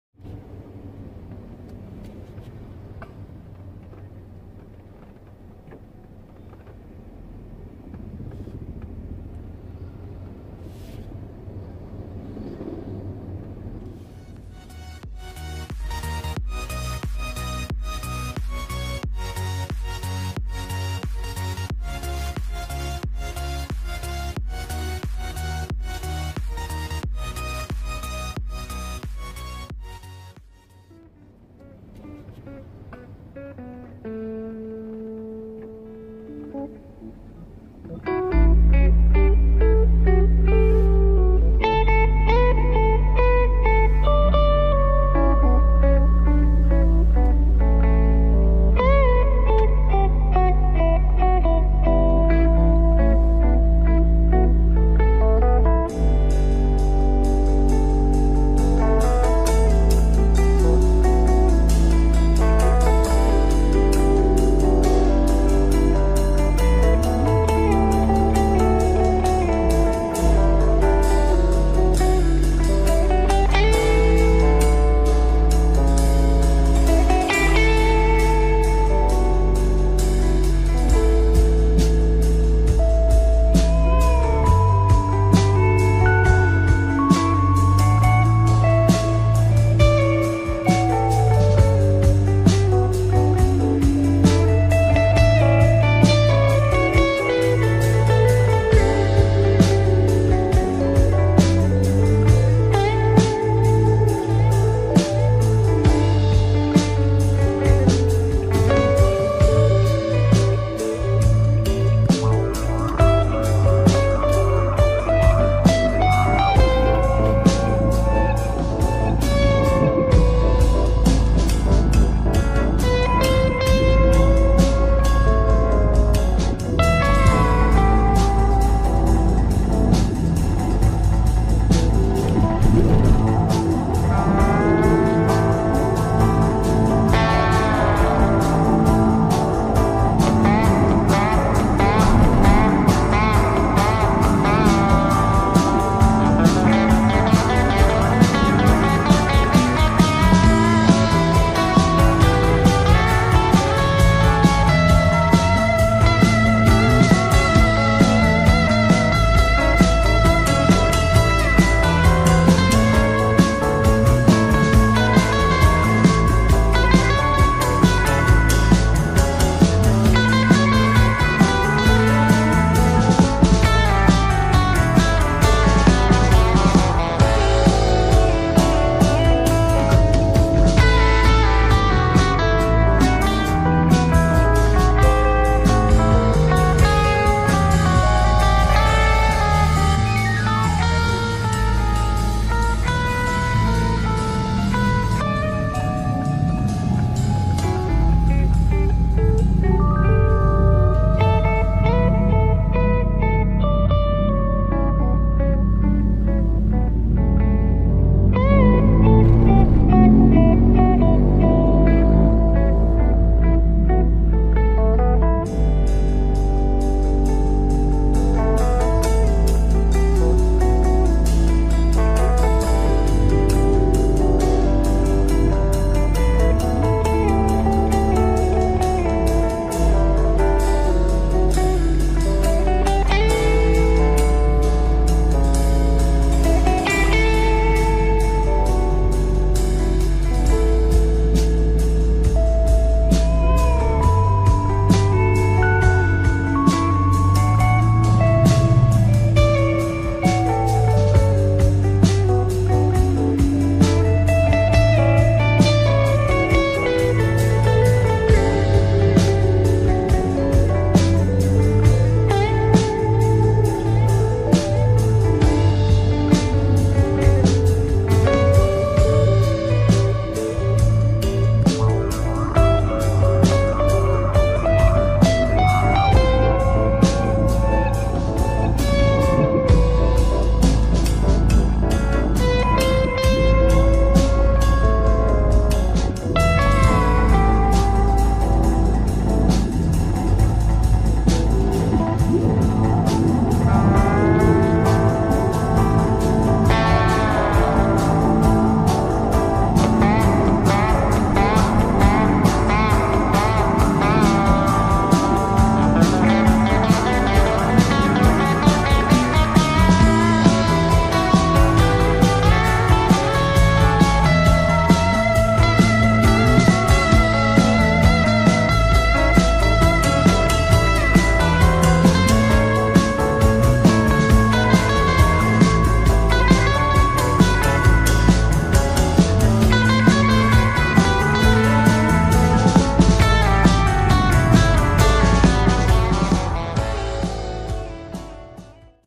Perjalanan Hidup
Kisah Kisah Ku Setiap Hari Banting Setir Siang dan Malam Mencari Rejeki Halal Barokah ... Pandemi Covid 19 Semoga Cepat Berlalu